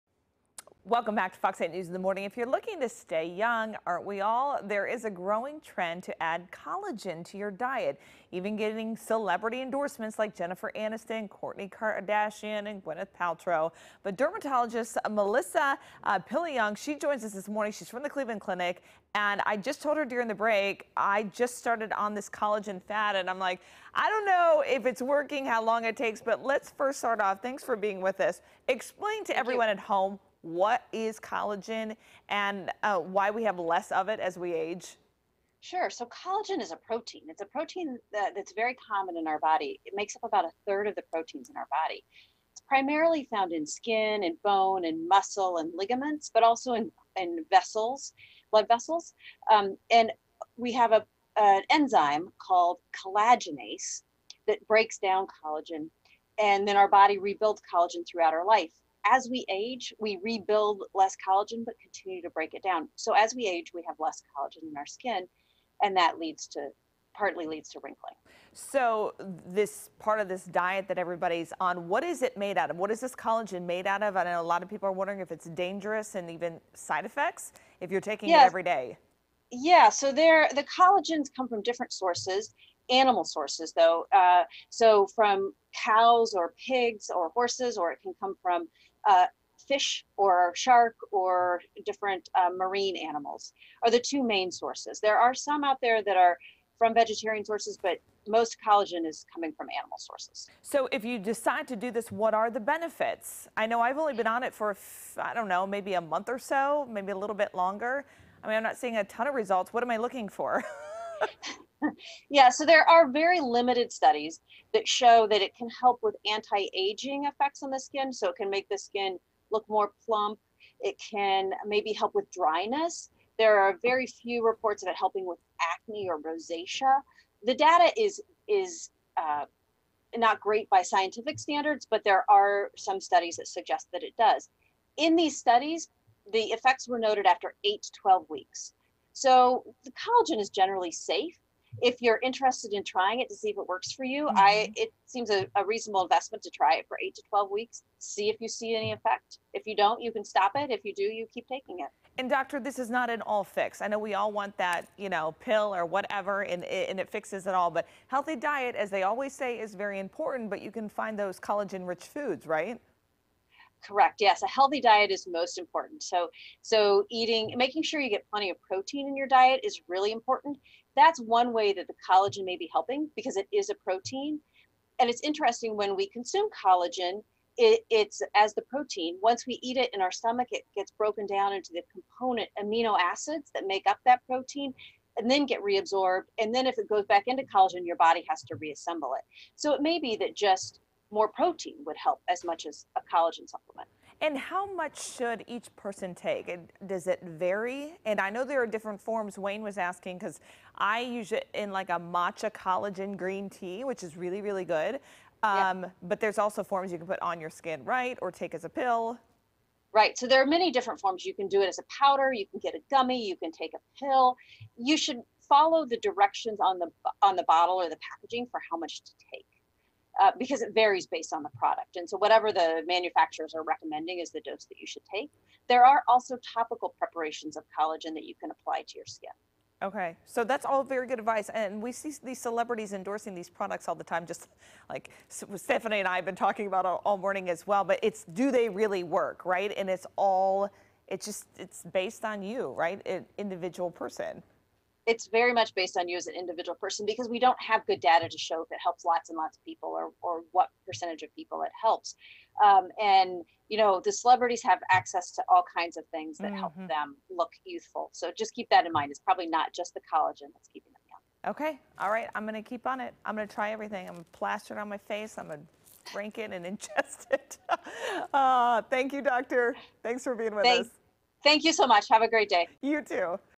Can collagen supplements reduce the signs of aging?
With many celebrities currently endorsing collagen supplements as a secret weapon to staying young looking, is it something we all should consider at as we age? Fox 8's Kristi Capel discussed the idea with Dr. Melissa Piliang, Dermatologist with Cleveland Clinic Hospitals.